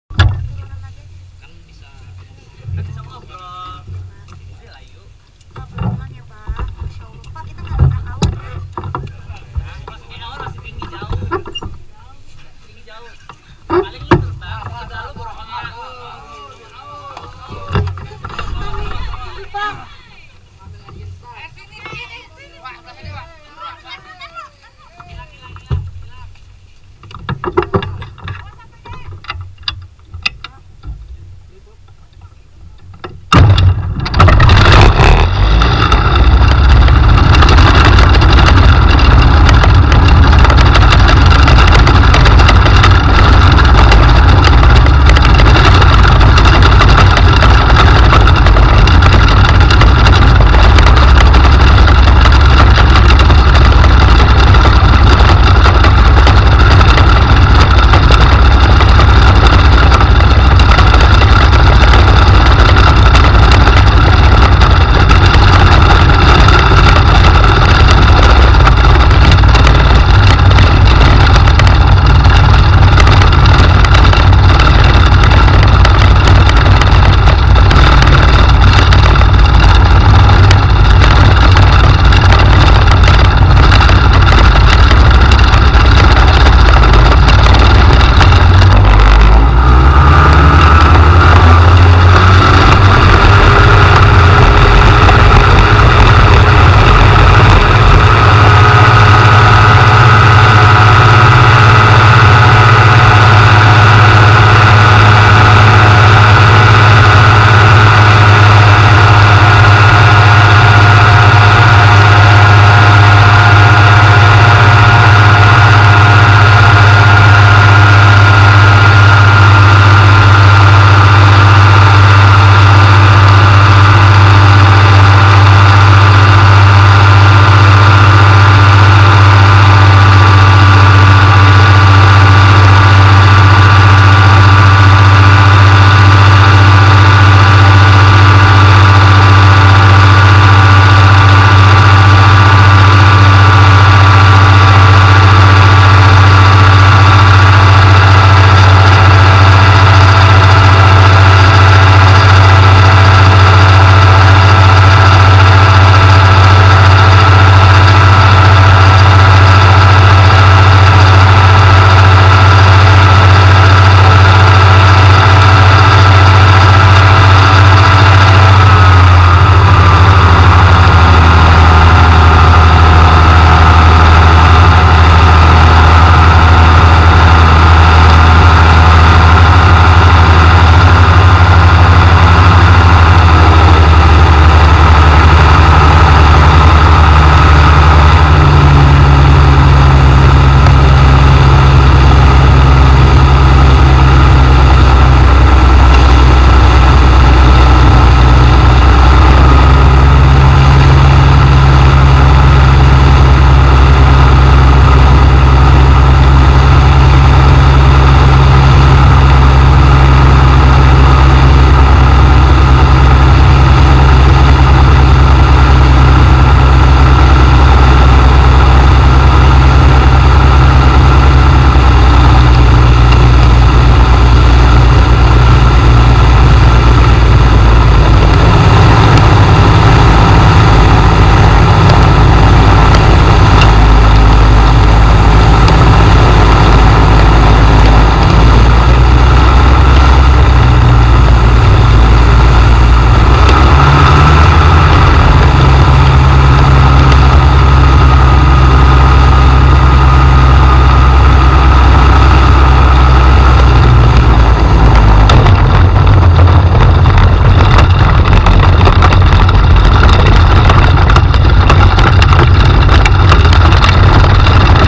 Ayu Ting Ting Terbang Melayang di Angkasa
Ayu Ting Ting Diva Dangdut Indonesia Terbang Melayang di Angkasa

LOKASI :
LIDO RESORT, Sukabumi
HANGGAR PEGASSUS, Sukabumi